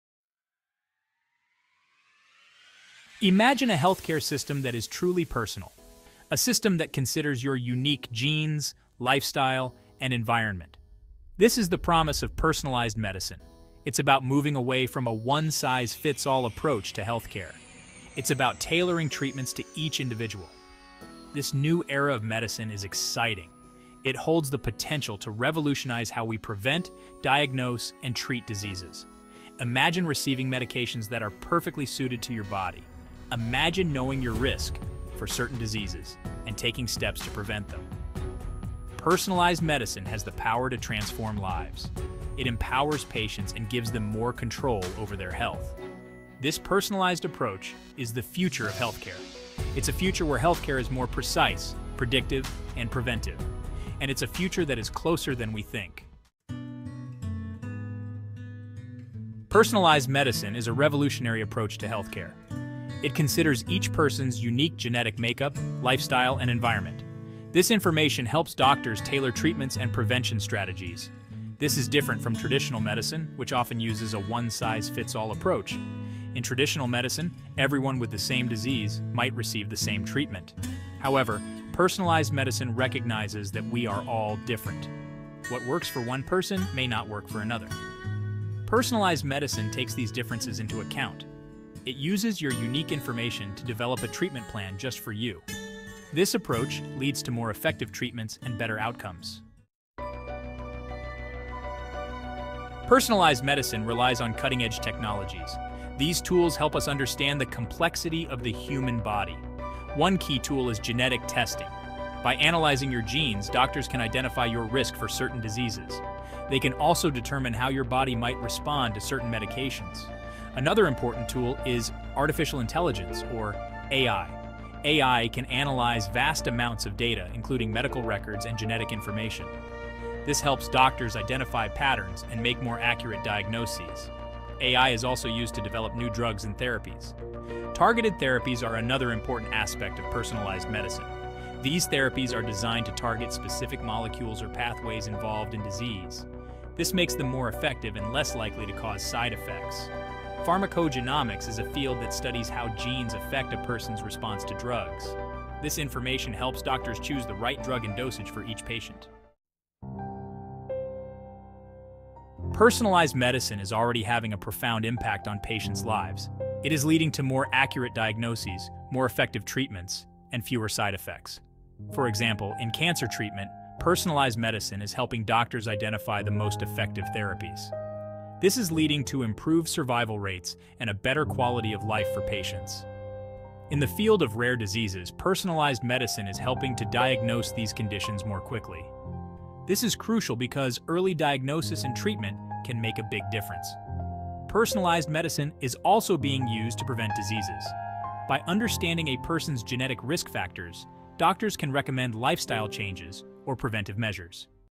2025 - Personalized Medicine: The Future of Healthcare
Personalized Medicine: The Future of Healthcare

Imagine a healthcare system that treats each person as a unique individual, tailoring treatments based on their genetic makeup, lifestyle, and environment. This approach, known as personalized medicine, is revolutionizing the field of healthcare by creating targeted therapies that are more effective, safer, and better suited to each patient.